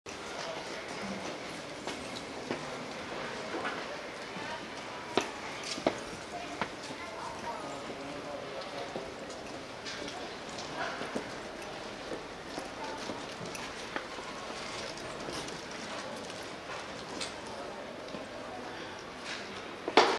20431a widm trailer vidEdi 20s hT
Venezia, Italia. Gondola on Rio de Ca´ Widmann. Venice is UNESCO World Cultural Heritage Site. Upright Size.